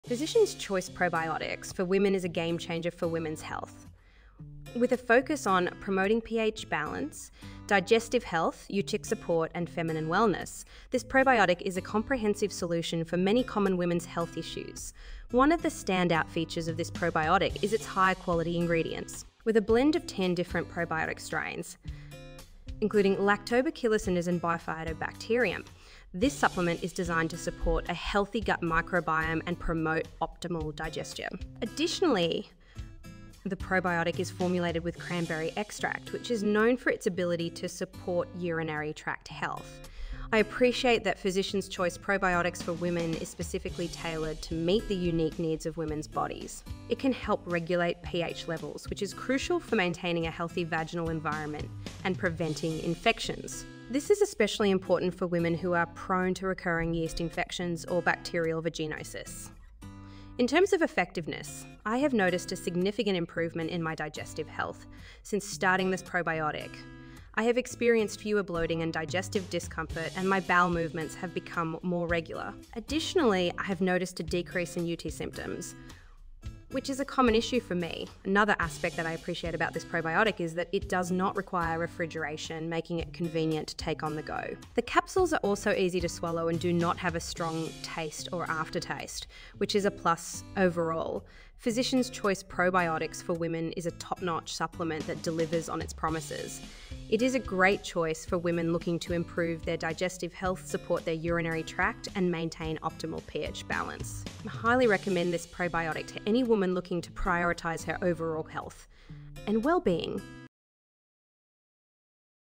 Physician's Choice Probiotics for Women PH Balance Digestive UT & Feminine Review
Physician's Choice Probiotics for Women is a game-changer for women's health. With a focus on promoting pH balance, digestive health, UT support, and feminine wellness, this probiotic is a comprehensive solution for many common women's health issues.

One of the standout features of this probiotic is its high-quality ingredients. With a blend of 10 different probiotic strains, including Lactobacillus and Bifidobacterium, this supplement is designed to support a healthy gut microbiome and promote optimal digestion. Additionally, the probiotic is formulated with cranberry extract, which is known for its ability to support urinary tract health.

I appreciate that Physician's Choice Probiotics for Women is specifically tailored to meet the unique needs of women's bodies. It can help regulate pH levels, which is crucial for maintaining a healthy vaginal environment and preventing infections. This is especially important for women who are prone to recurring yeast infections or bacterial vaginosis.

In terms of effectiveness, I have noticed a significant improvement in my digestive health since starting this probiotic. I have experienced fewer bloating and digestive discomfort, and my bowel movements have become more regular. Additionally, I have noticed a decrease in UTI symptoms, which is a common issue for me.

Another aspect that I appreciate about this probiotic is that it does not require refrigeration, making it convenient to take on the go. The capsules are also easy to swallow and do not have a strong taste or aftertaste, which is a plus.

Overall, Physician's Choice Probiotics for Women is a top-notch supplement that delivers on its promises. It is a great choice for women looking to improve their digestive health, support their urinary tract, and maintain optimal pH balance. I highly recommend this probiotic to any woman looking to prioritize her overall health and well-being.